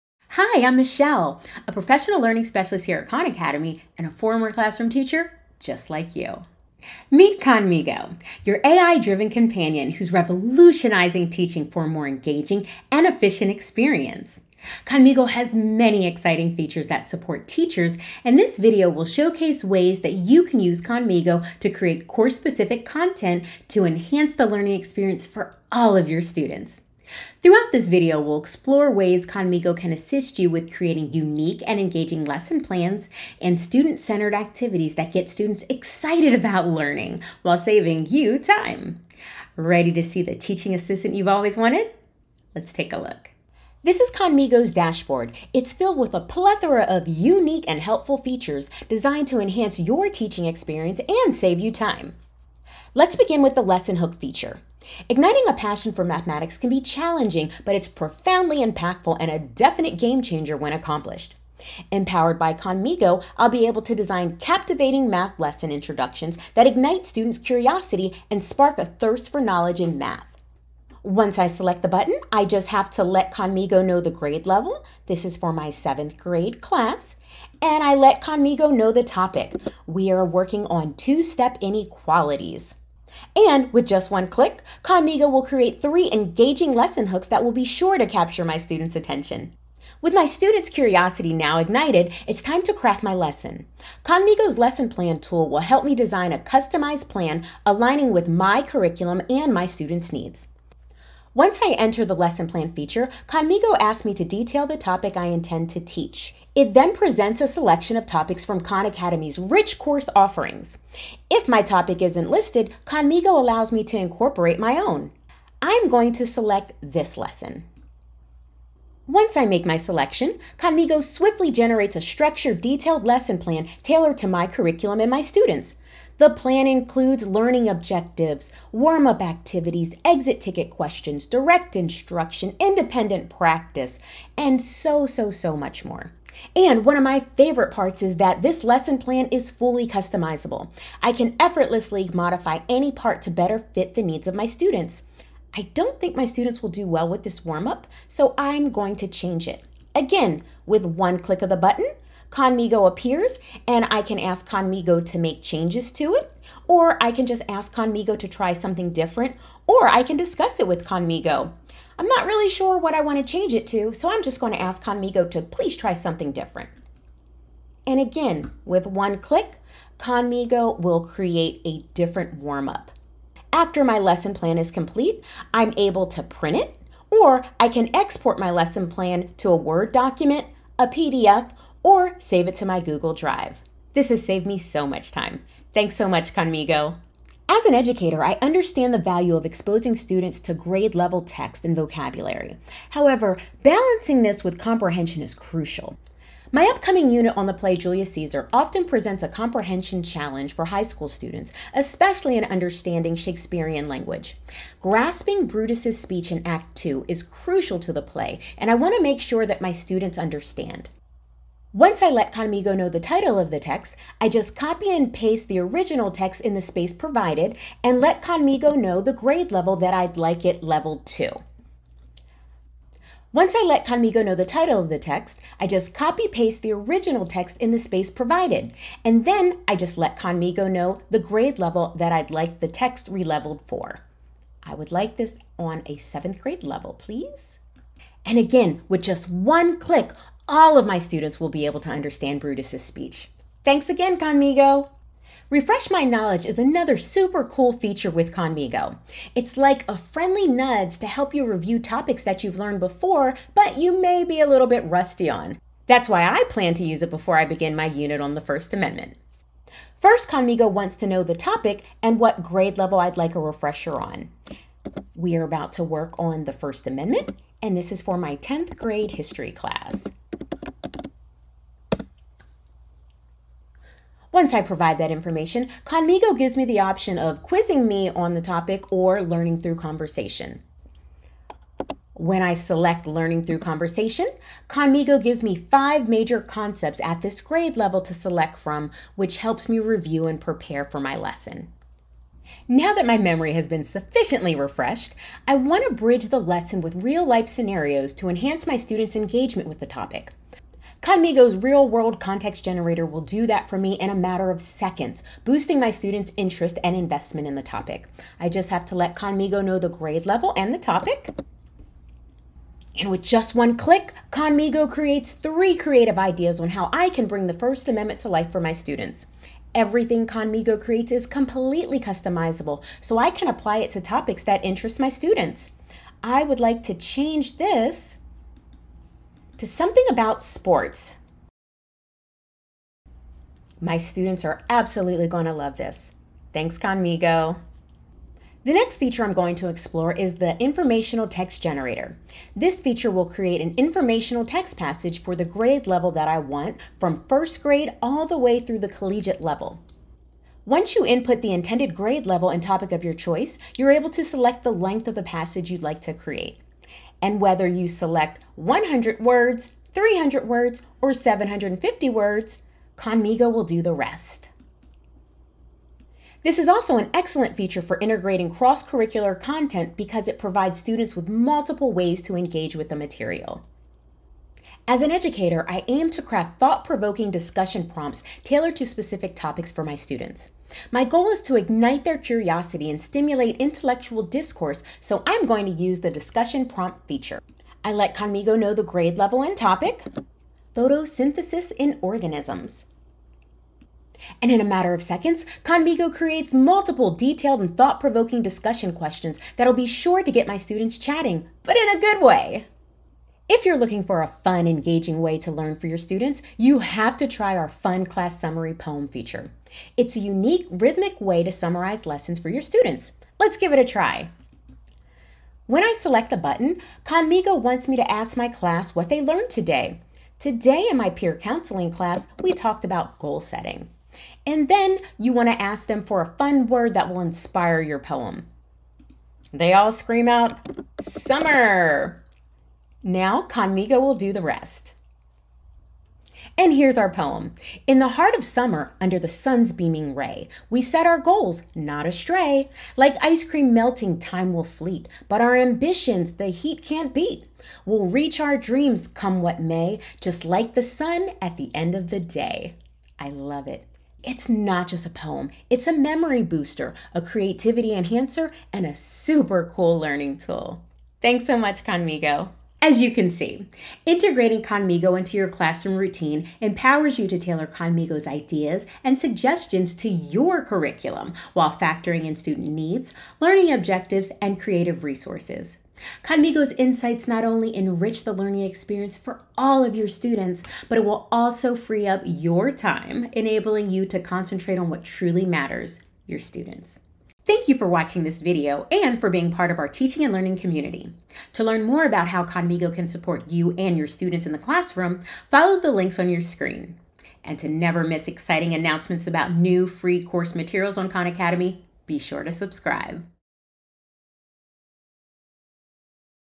Khanmigo for Teachers
Khan Academy's AI-powered teaching assistant and student tutor, Khanmigo, has been upgraded with brand new features that are making it so much easier for teachers to reach their students where they are. Join Khan Academy's professional learning specialist and former classroom teacher, Michele, to see how Khanmigo can supercharge the learning in your classroom.

For more from Khanmigo, visit: khanmigo.ai

Contents
00:00 - Introduction to Khanmigo
00:50 - Create a Lesson Hook
1:31 - Create a Lesson Plan
3:07 - Leveler Feature
4:05 - Refresh My Knowledge
4:52 - Real-World Context Generator
5:39 - Informational Text Generator
6:14 - Discussion Prompt Generator
6:43 - Fun Class Summary Poem Feature
7:43 -  More from Khanmigo